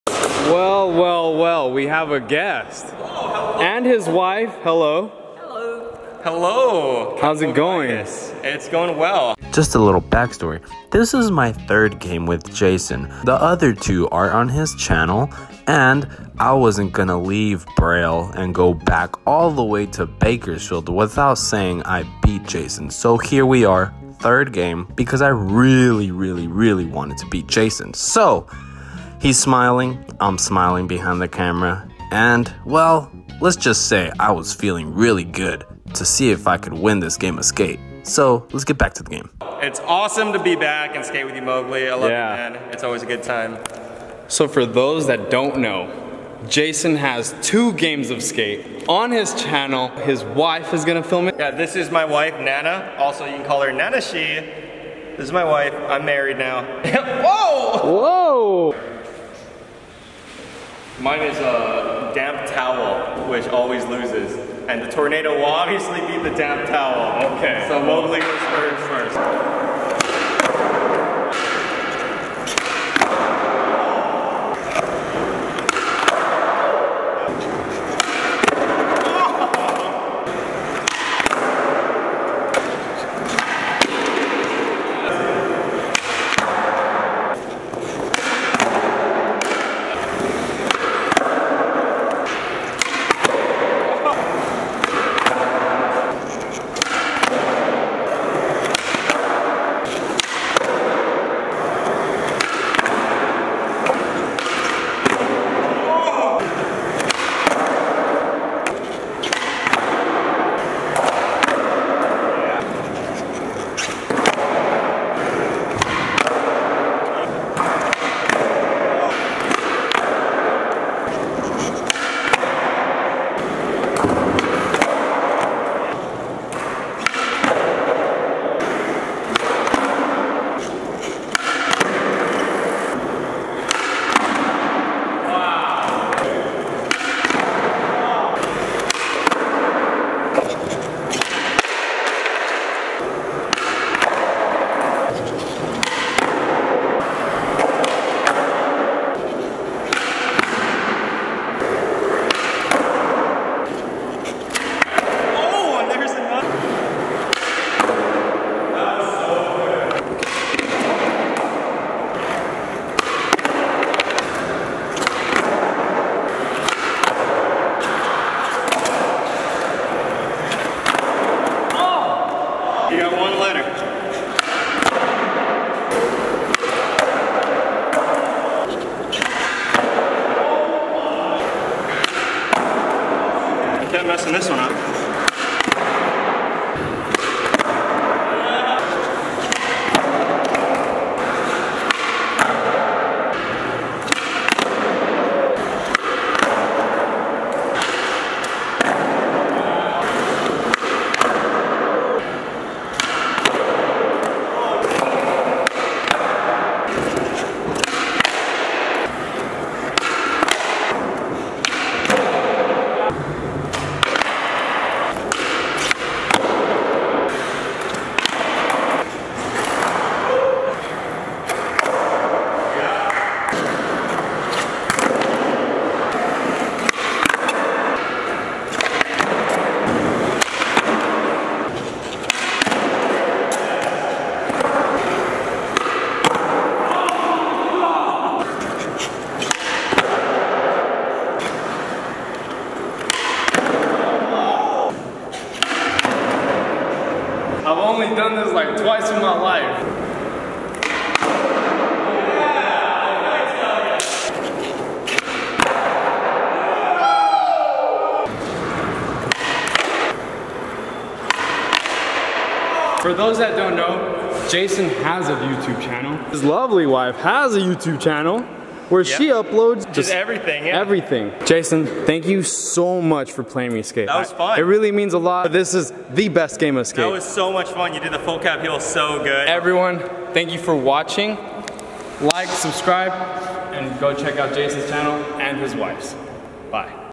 BEST GAME OF SKATE I'VE PLAYED
in this video I play Jason park what seemed to be one of the best games I've ever played hope y'all enjoy.